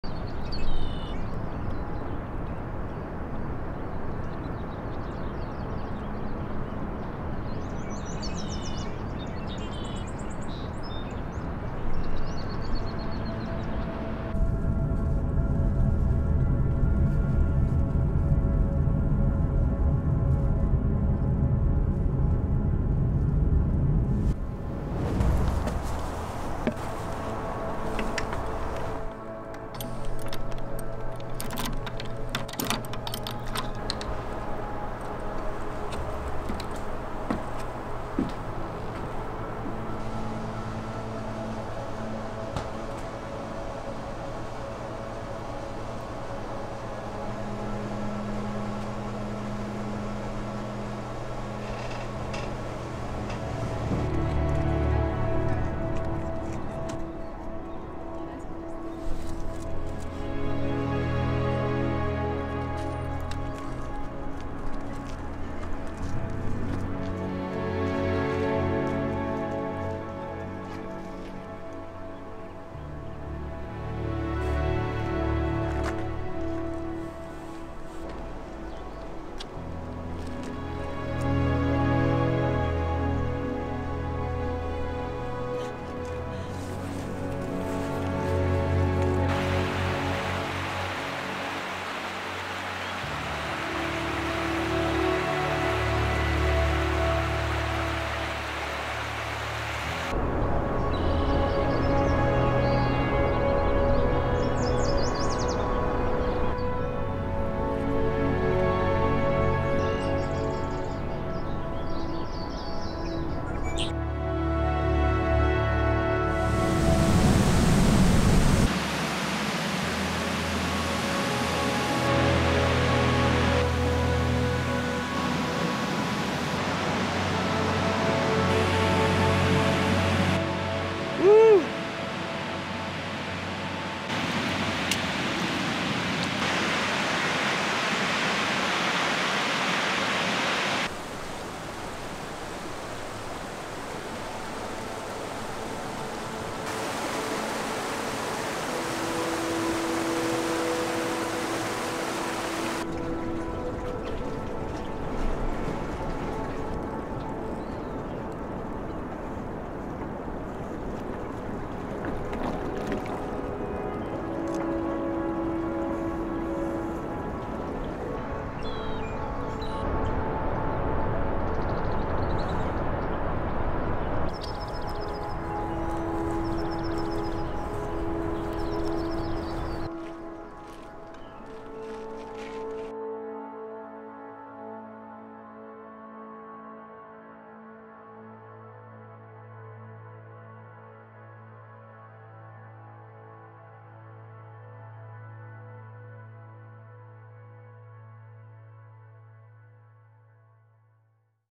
Yosemite Spring 2019 Sony A7iii 4K | Ambient Road Trip | Focus, Sleep, Relax, Study
I've been thinking about Yosemite a lot lately. This might be the first time in 10 years that we won't be visiting the park in Spring. Wanted to remember the magical moments from last spring with this short video. Hope you are all safe and healthy.

Shot all handheld, 4k 24P (1-2 scenes 1080p 120fps), S-log 2

Equipment Used:

Lens 1
Lens 2
Lens 3